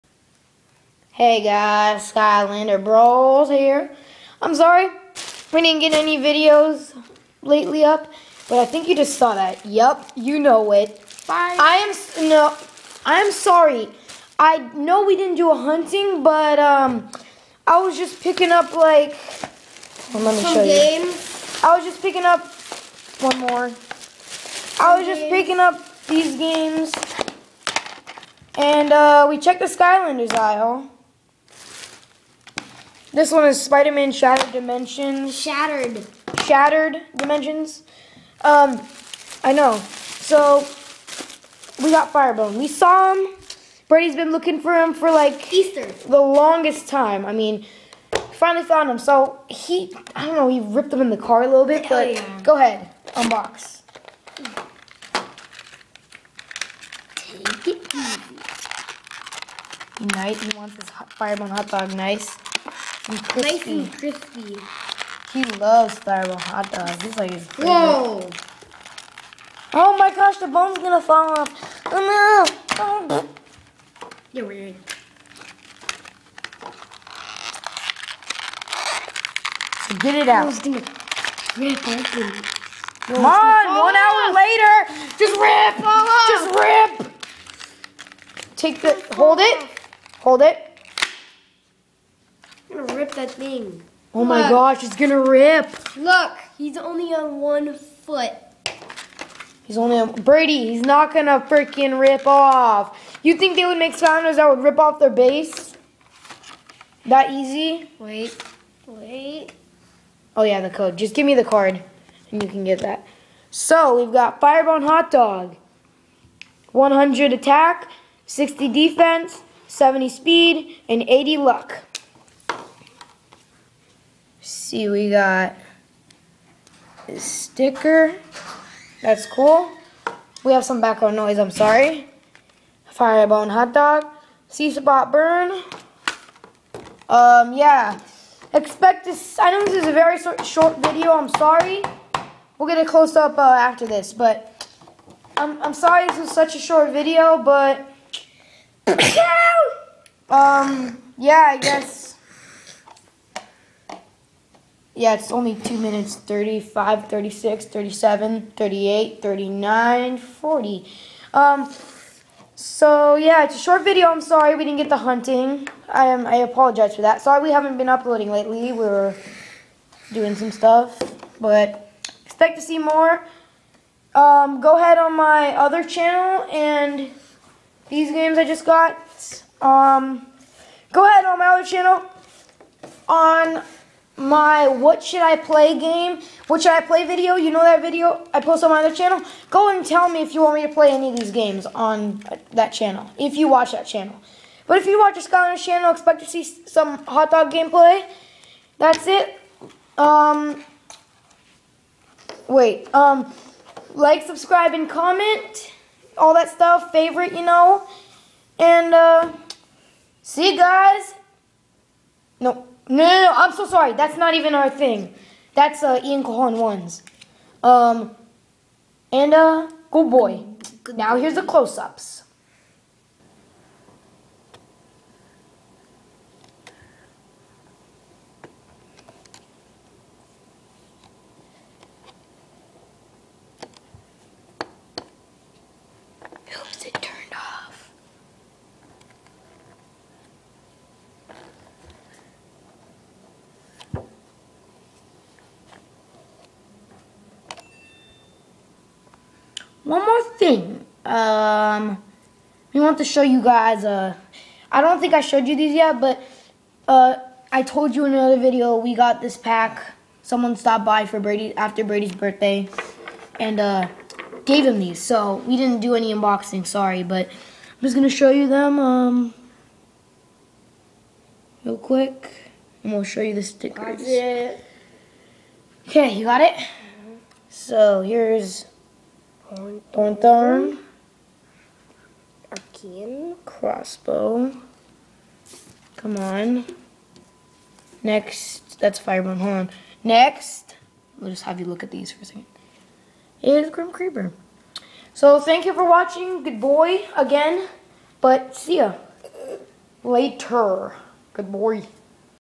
Skylanders-Fire Bone Hot Dog Unboxing
In this video we unbox Fire Bone Hot Dog, sorry for no hunting